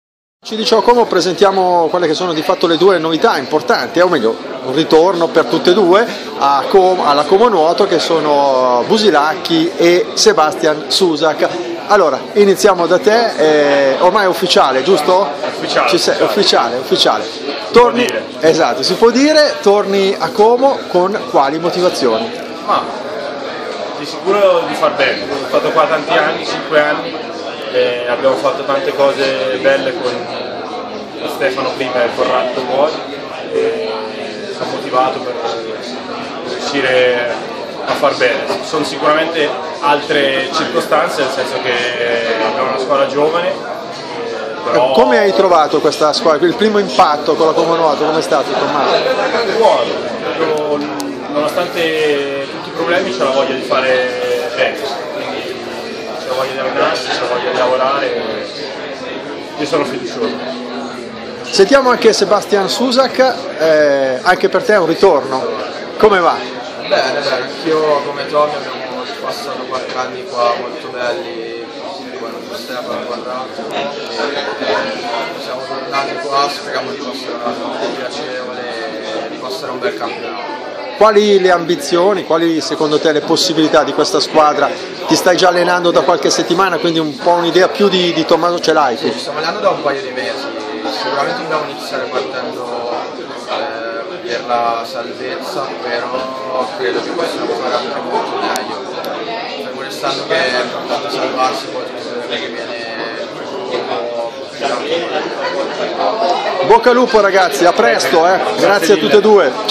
BUSILACCHI E SUSAK, ECCO I DUE GRADITI RITORNI IN COMONUOTO
Le prime impressioni di Tommy dopo aver ufficializzato l'ingaggio, il difensore ritorna al club che lo ha lanciato